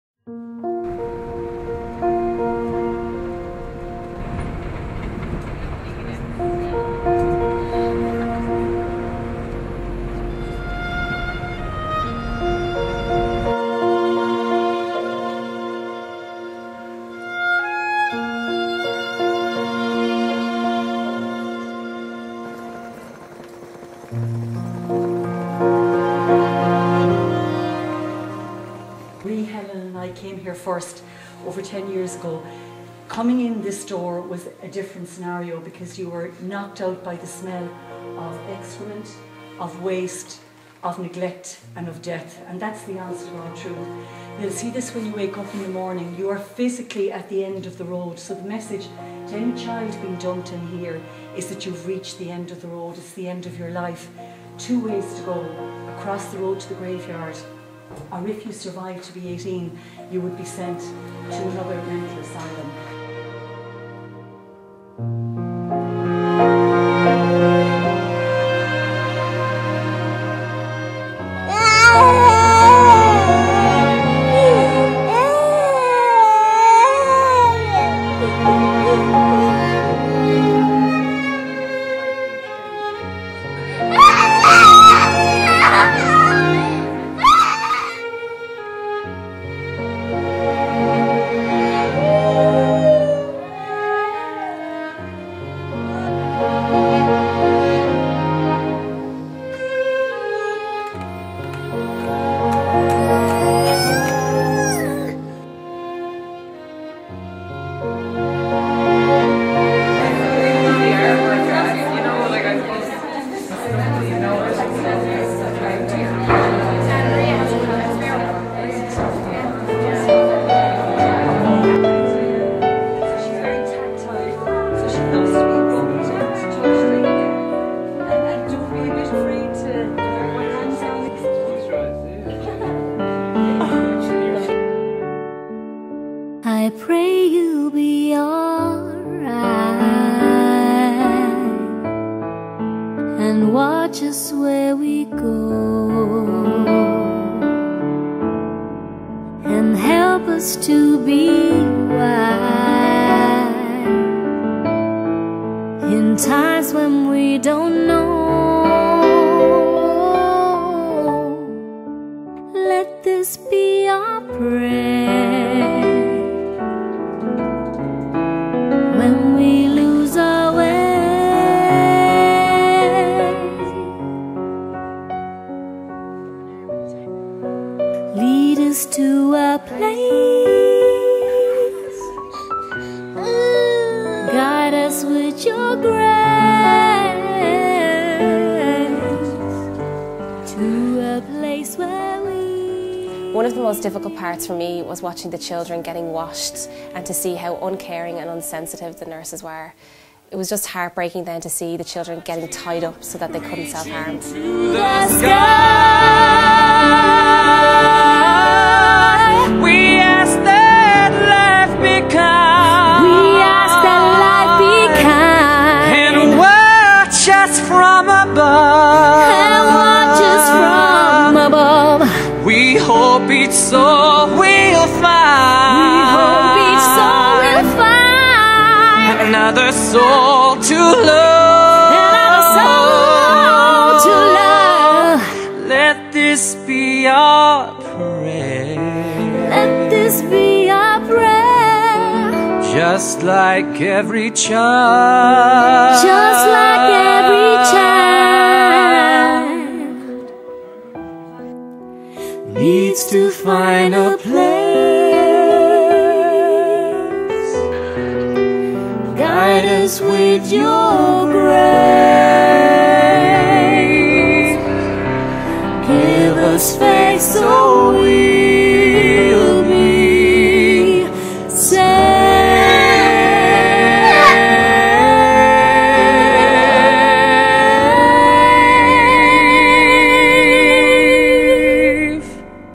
Chernobyl Vesnova kids mental asylum
Footage from Irish Rose of Tralee trip to VESNOVA Orphanage in Belarus with Adi Roches Chernobyl Children International